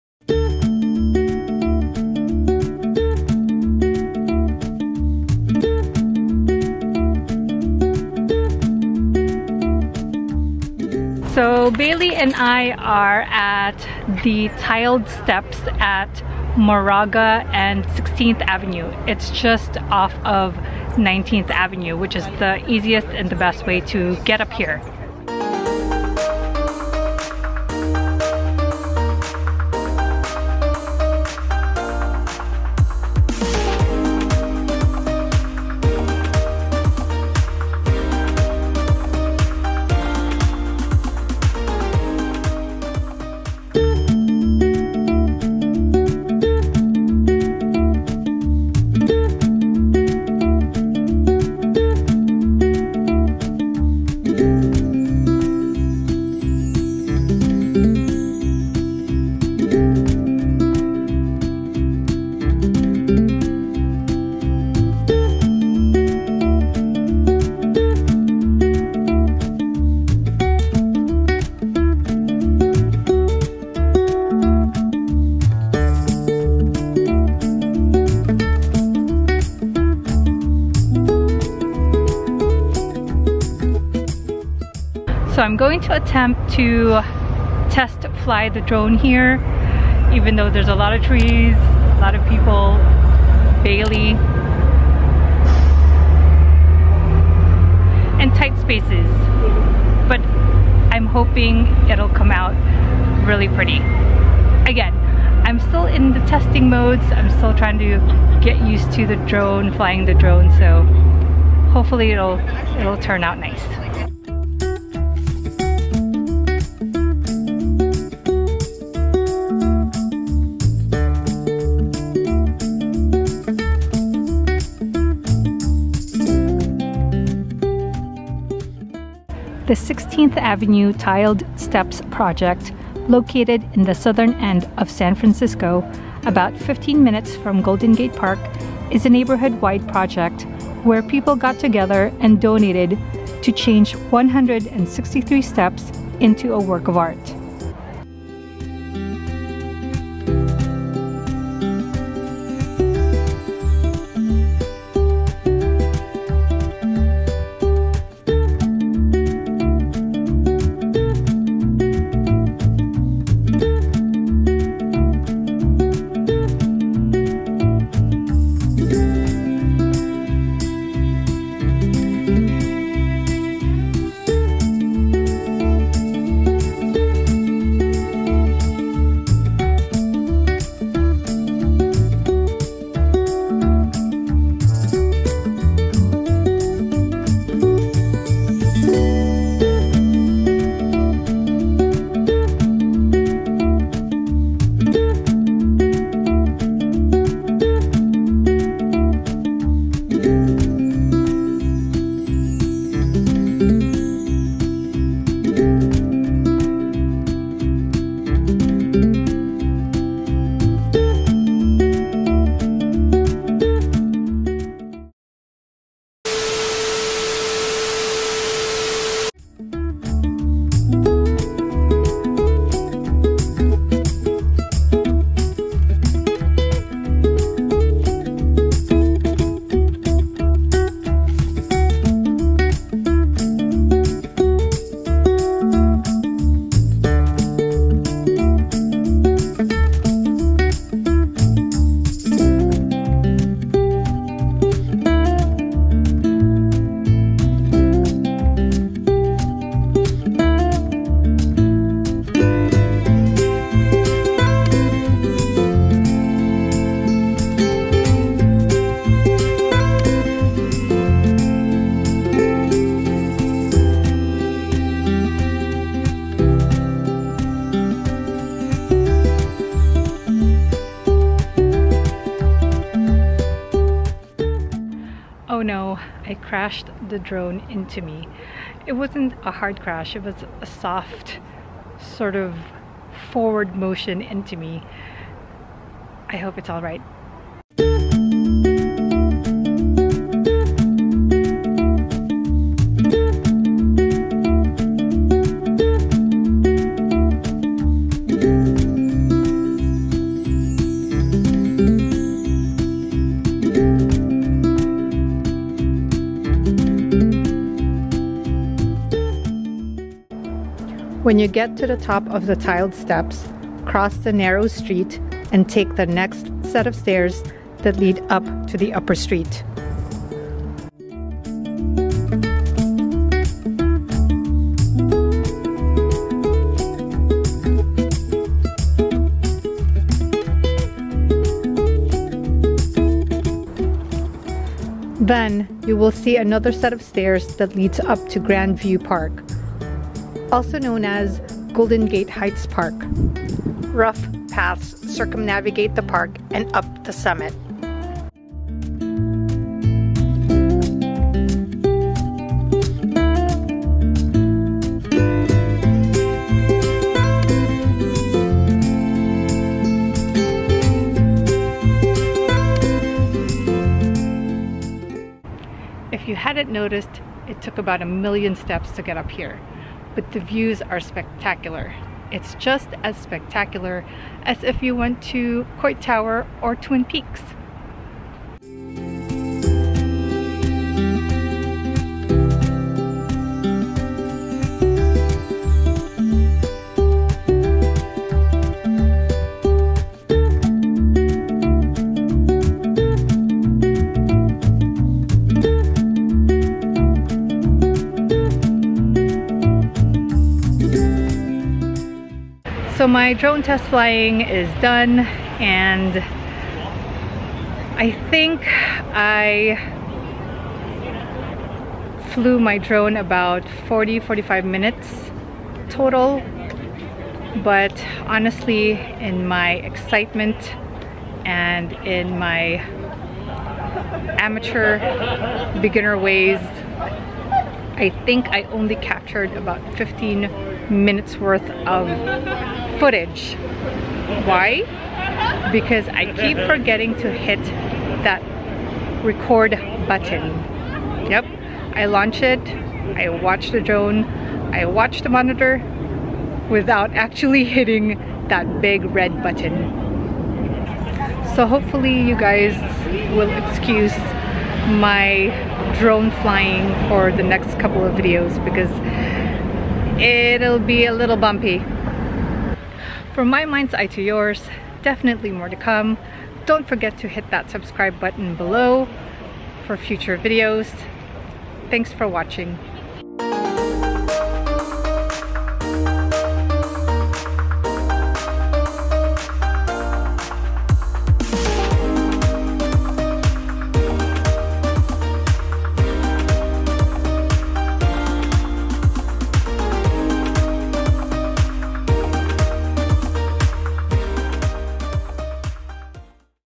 48 hours of Flying the Mavic Air - Hidden Gem: Mosaic Steps - San Francisco, Ca - February 2019
The drone is intact🥳 My brain-to-hand coordination is bad😖 I fail to remember many pre-flight checks, even the basic ones like taking the lens protector out, extending the leg antennae, attaching the joysticks before sliding in my phone, or worst yet… forgetting to hit the record button😥 On a good note, The battery🔋life is actually forgivable, in that, I may only need 15-20 mins of flight time in an area to capture useable angles. For now, a beginner like me, I’m burning through 2.5 batteries in an area which includes getting worked up just trying to maneuver it around😤 Although my drone seems to be a fragile device😏 it is definitely a fun tool and a great addition to my gadgets.

Little did I know that San Francisco doesn’t want drones in parks and that the San Francisco Recreation and Park Department are issuing citations of up to $200 for flying drones in city parks. I read about it AFTER my test flight in Golden Gate Park, but the Tiled Steps is not considered a park, either way, thankfully, I got away with it😳

The SF Rec and Parks does allow the commercial use of drones with a permit from the Film Permit Office sine drones fall under “Machine or apparatus of aviation”.

Now, I have to find open spaces in the bay area where I can continue to test fly and get used to flying the drone🤷🏻‍♀️

Camera Equipment:
DJI Mavic Air Quadcopter with Remote Controller
Canon G7X Mark II PowerShot
JOBY GorillaPod Original Tripod for Point and Shoot Cameras